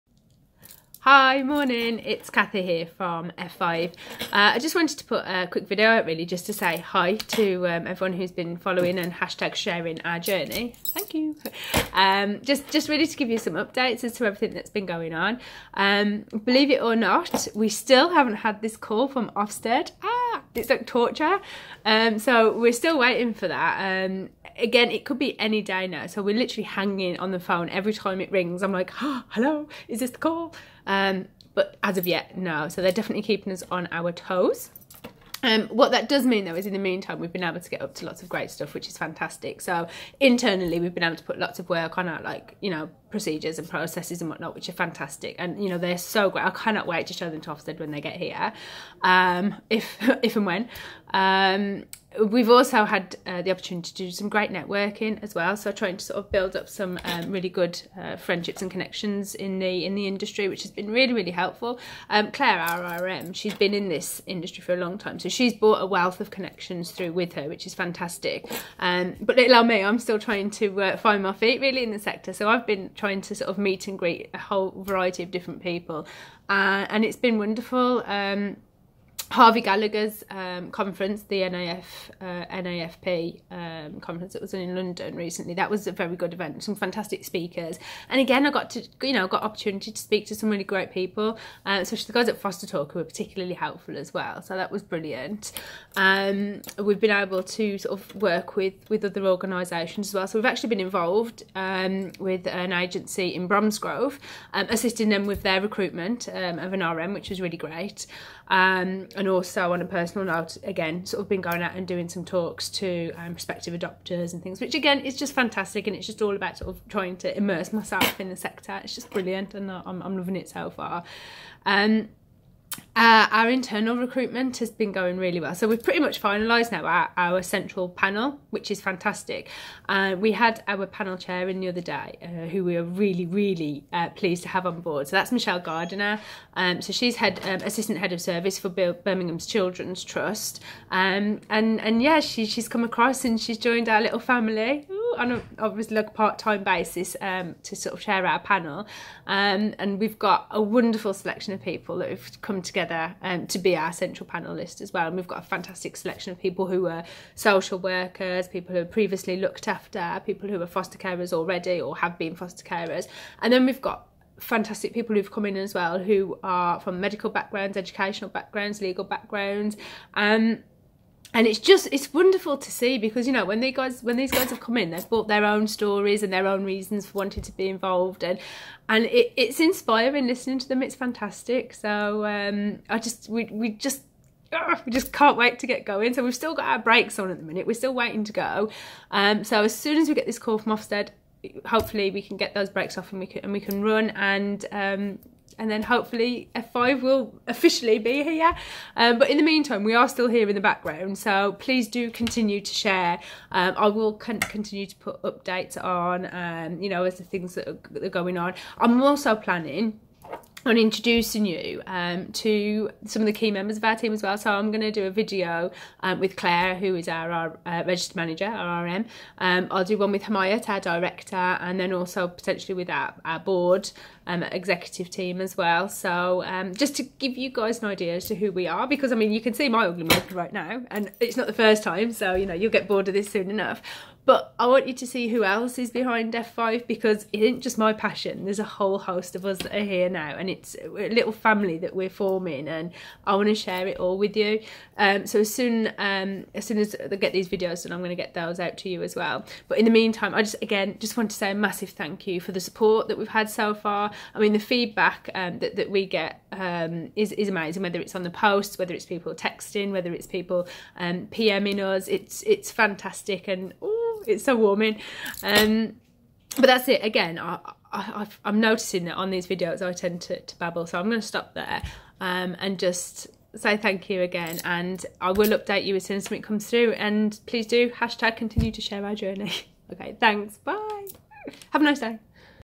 Pre Ofsted 2- Ofsted keeping us on our toes
Update video for our F5 Foster Care followers - still no news from Ofsted (aarrrgghh the torture of the wait) lots going on though behind the scenes getting things sorted and in place.